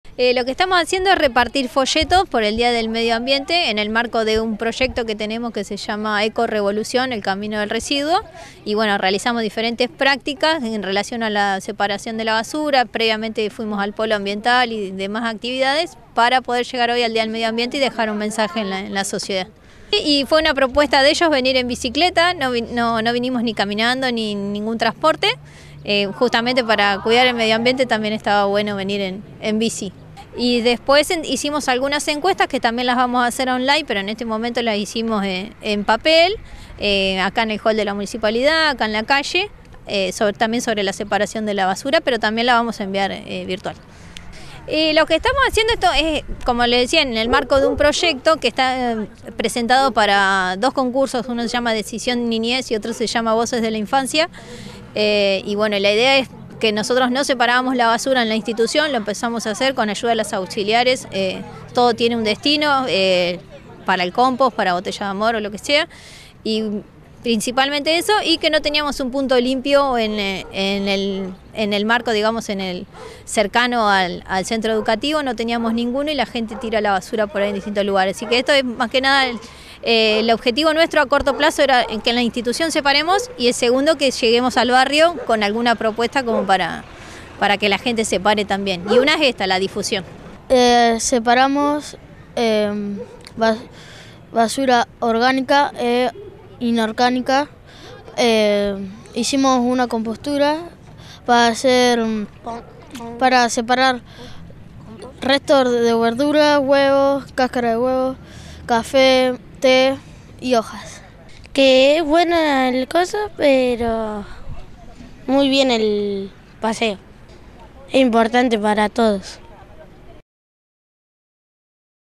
ACCIONES DEL CEC N° 801 POR EL DÍA DEL MEDIO AMBIENTE
FERNANDA SAAVEDRA Y ALUMNOS CEC 801 - Por un proyecto educativo presentado en dos concursos, el CEC 801 entrega folletos y realiza encuestas presenciales y virtuales para concientizar sobre el medio ambiente y su cuidado. El proyecto es más amplio y apunta a concientizar a la institución, las familias y el barrio